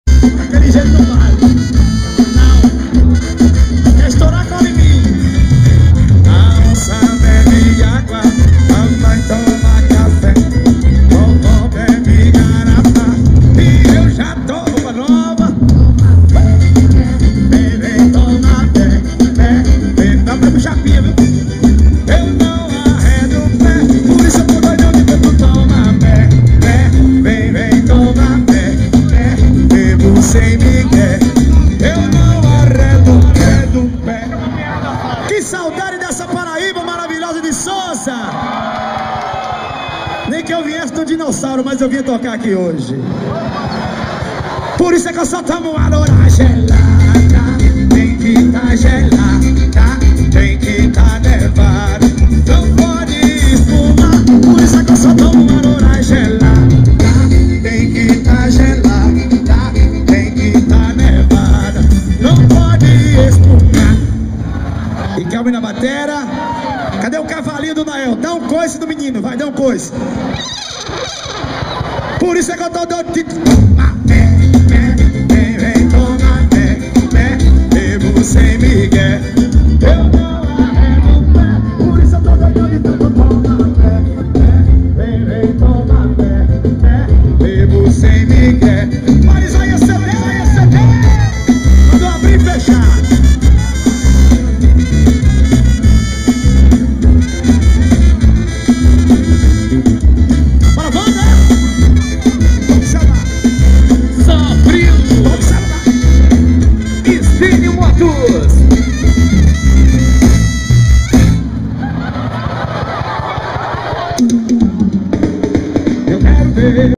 Paredão Lobo Mau no CTN
Antes do Show de Garota Safada Realizado no Centro de Tradições Nordestinas CTN Limão Dia 11/05/2012
Gravação Jefferson Lima Taffarel Silva ...